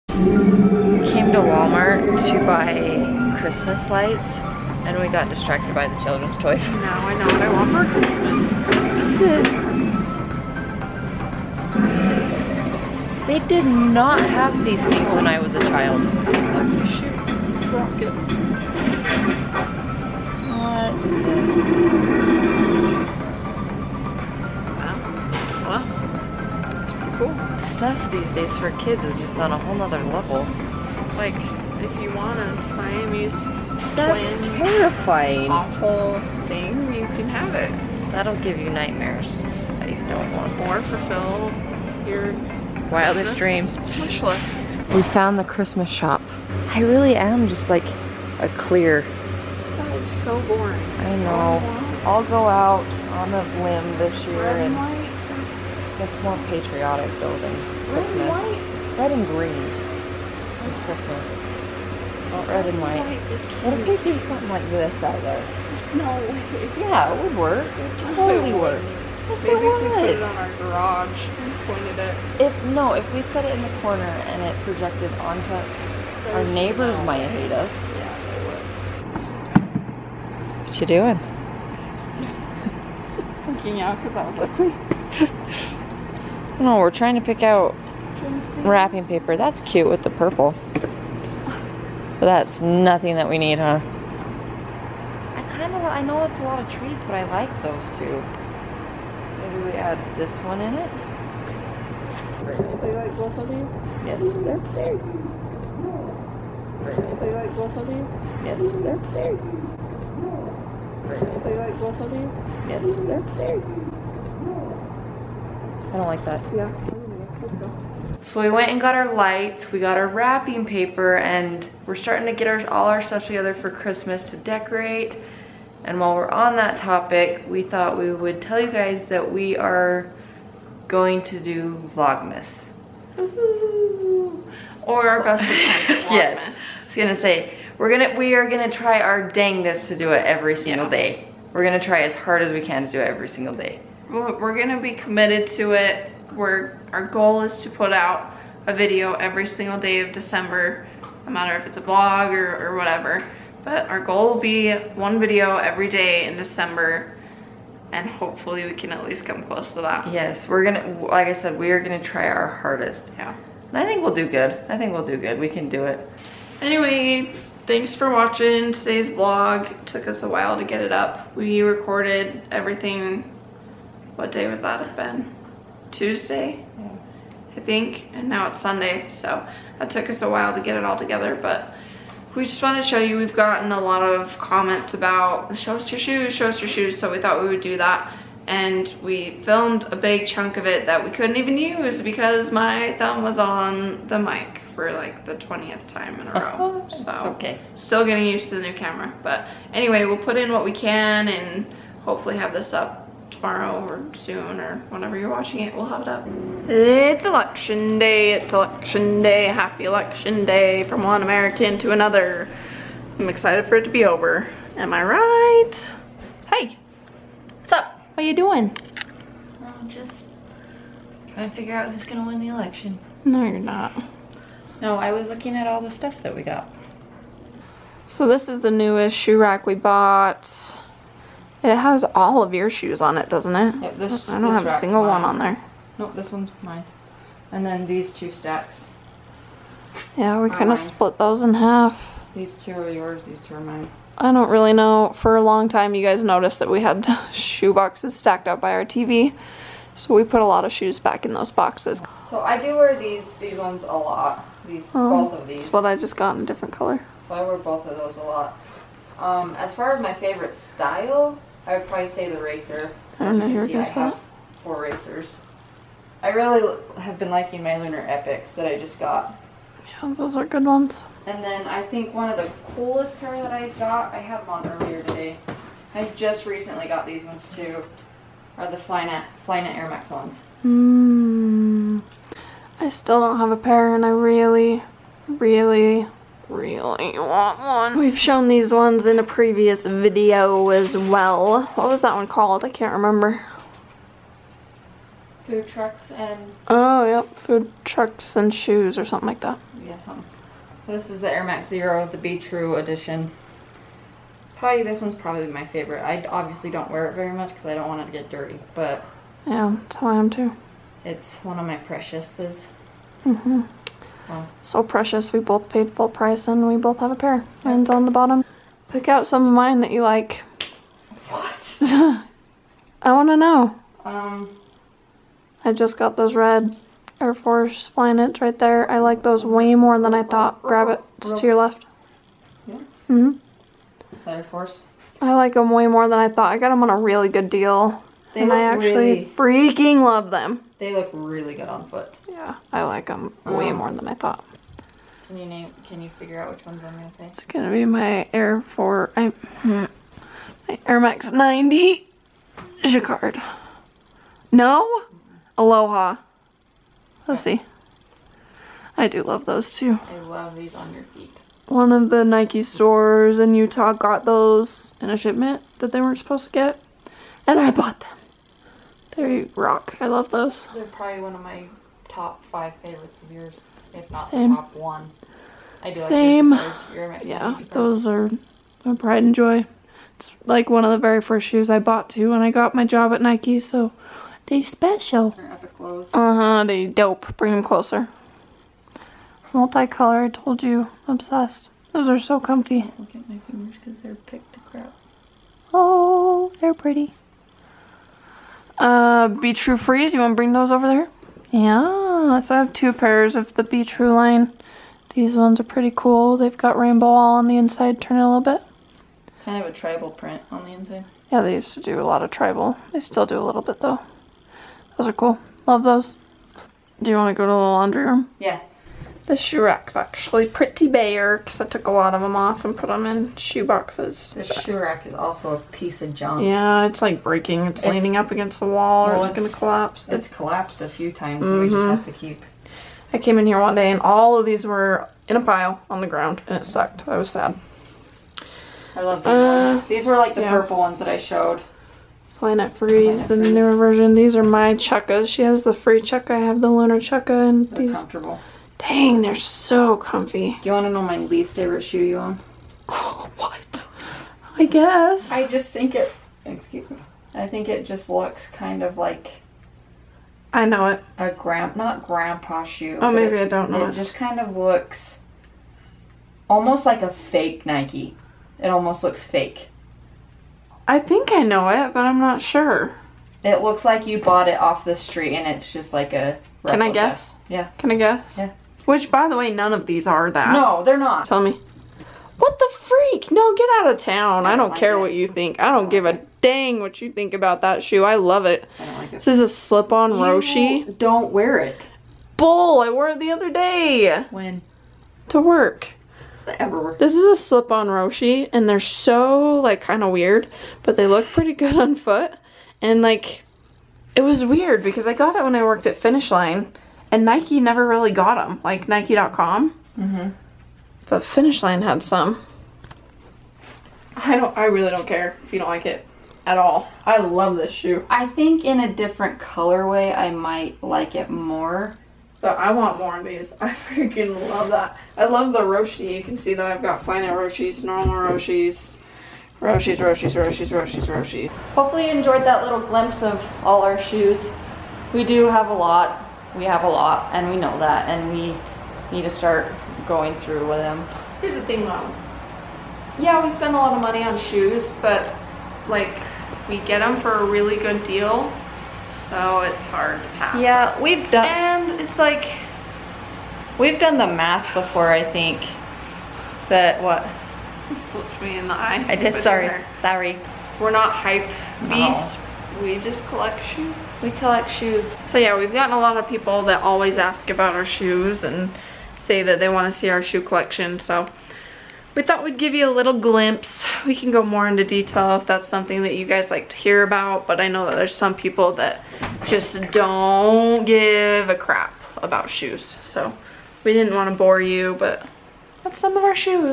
SHOE TOURS!
Couple things! 1) Lots of people have asked to see our shoes, so we gave you a little snippet. 2) Walmart is scary AF and scared us really bad! 3) WE DOIN VLOGMAS!!
*Views and opinions expressed are solely ours and not expressed on behalf of Nike.